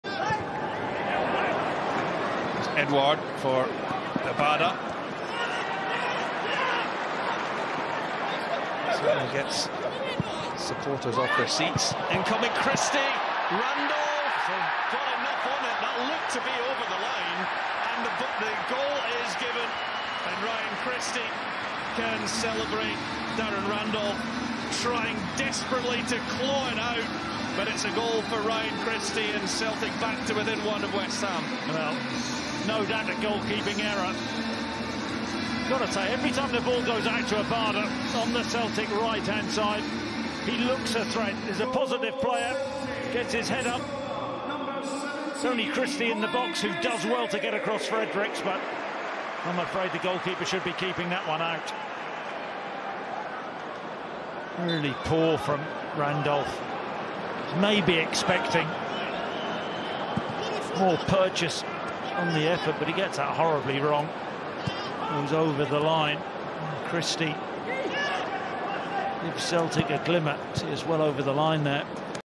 Ryan Christie gets on the scoresheet for Celtic in friendly against West Ham
Ryan Christie claws one back for Celtic! 🍀

Darren Randolph won't be pleased with his efforts to keep that out of the West Ham goal 👀